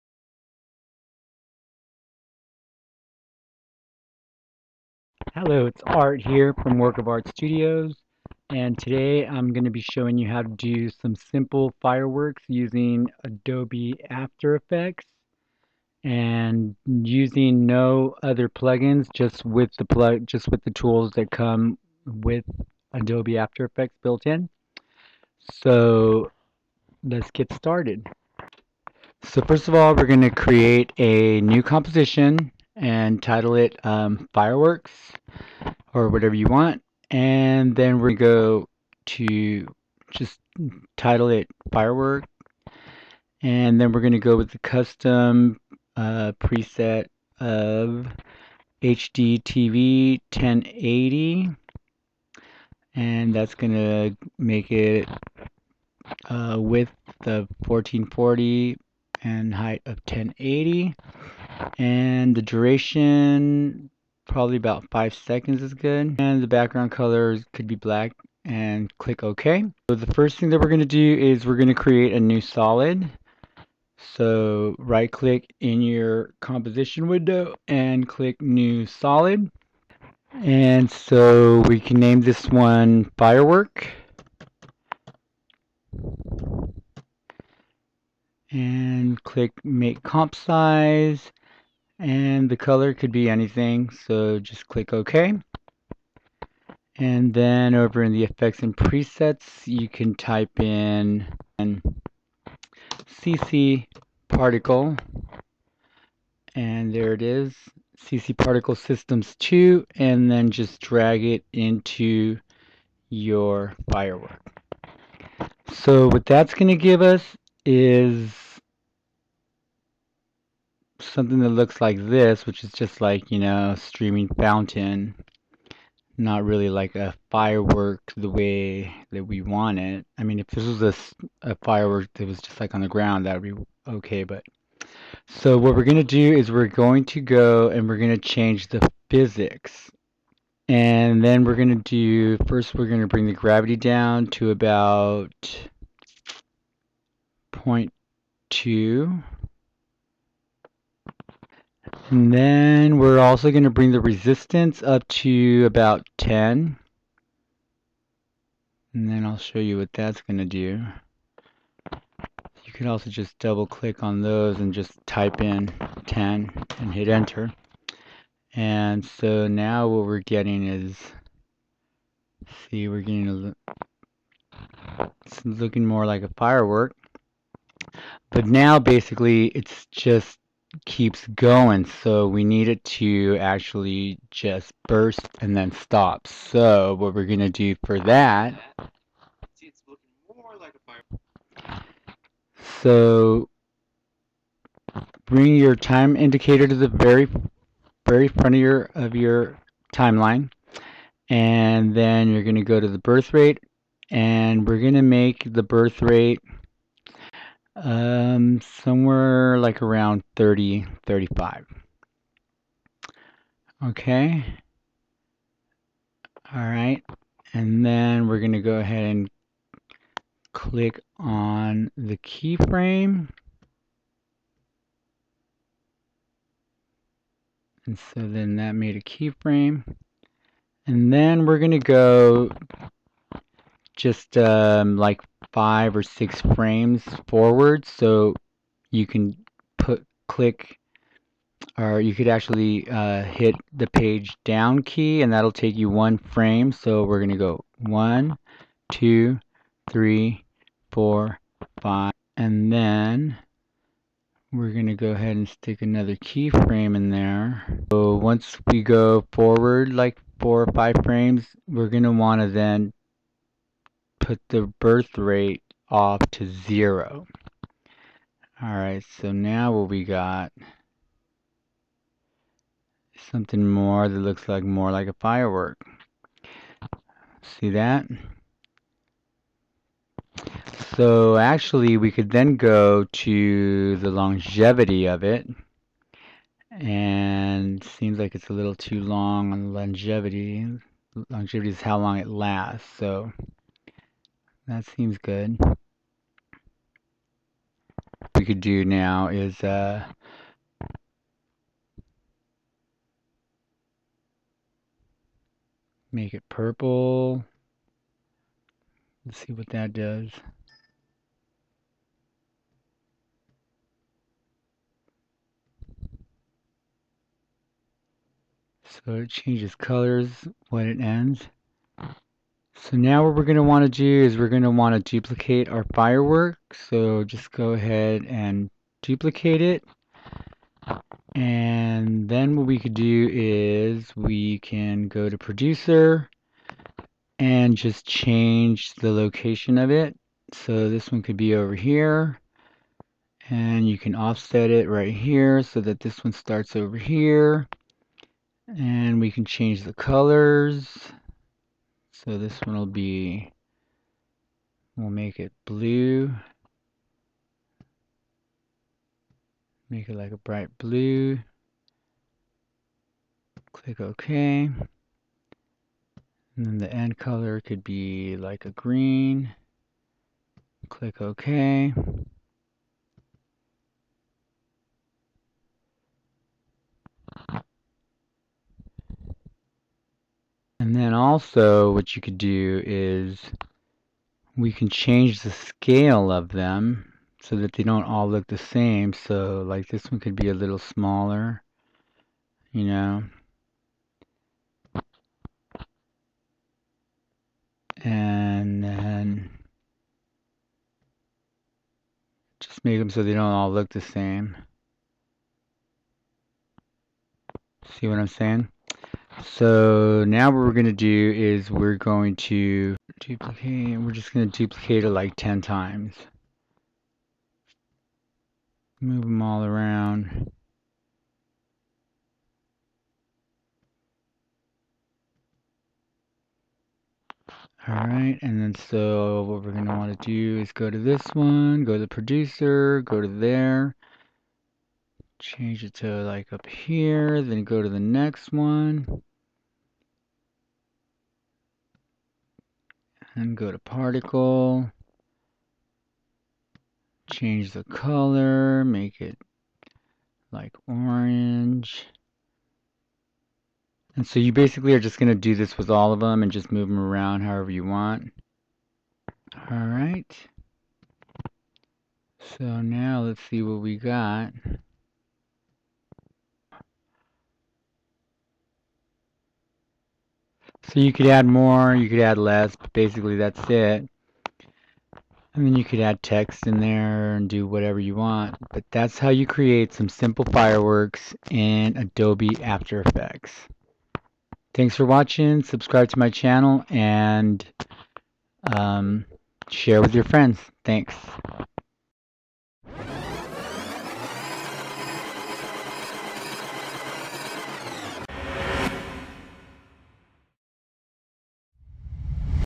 After Effects - Fireworks with out particular tutorial
In this video I will be showing you how to create fireworks with adobe after effects without using the plug-in Trapcode Particular,